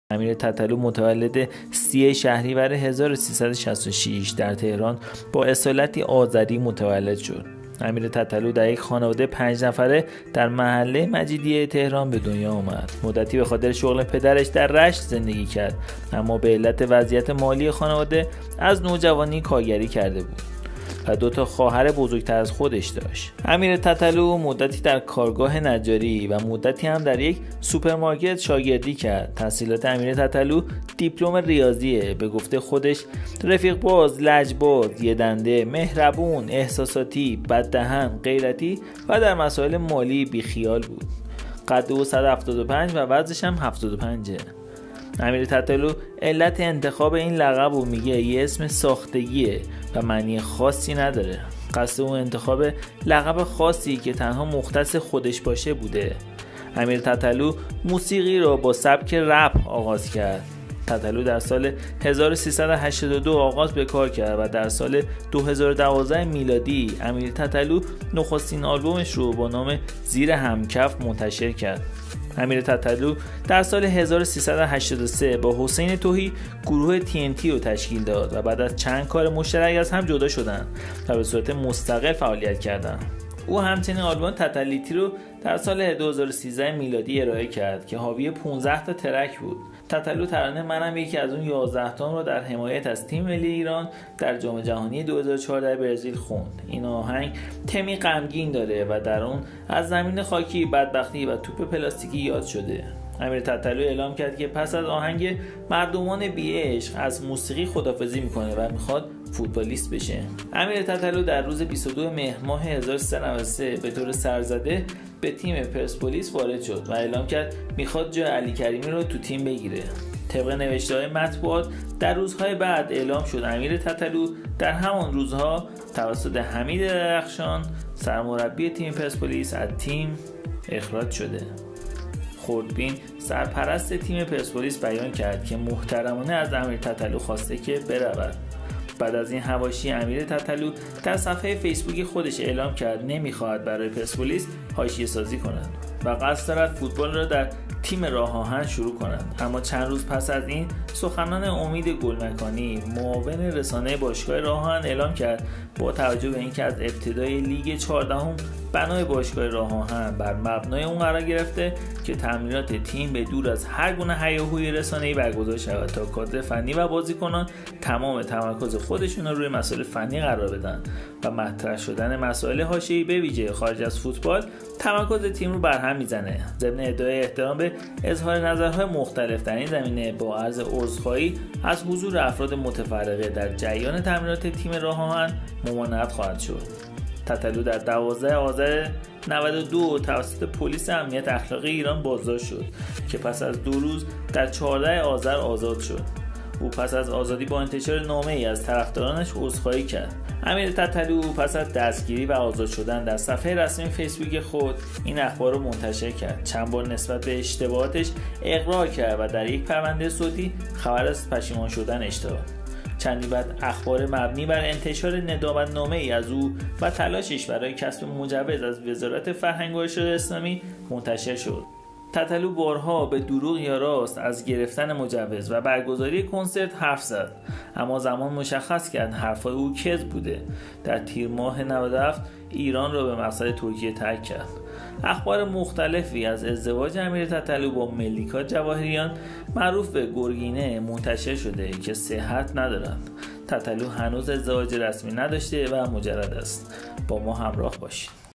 امیر تتلو از مجیدیه تا ترکیه
به دلیل خانواده ضعیفی که داشت از نوجوانی به کارگری مشغول شد